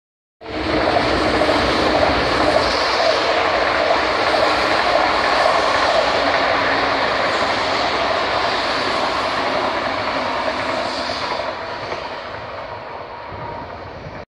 Trenes de la C-3 concidiendo en su paso por Pinto
El tren con destino Aranjuez coincide con el que se dirige a El Escorial a su paso por Pinto.